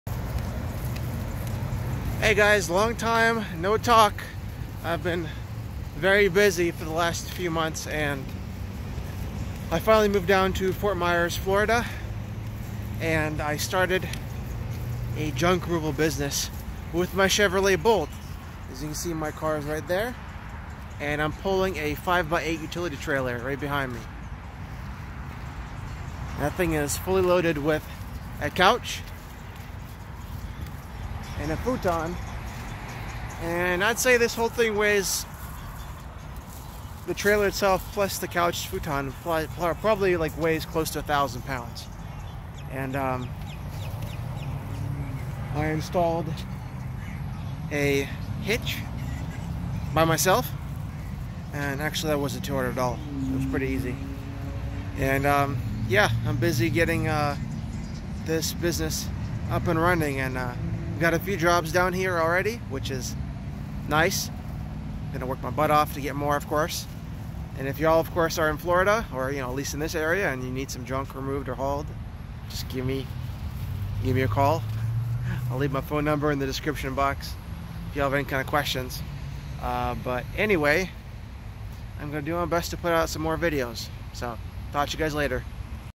The Chevy Bolt EV can TOW
I moved to Fort Myers and started my junk removal business, using my BOLT as the main tow vehicle.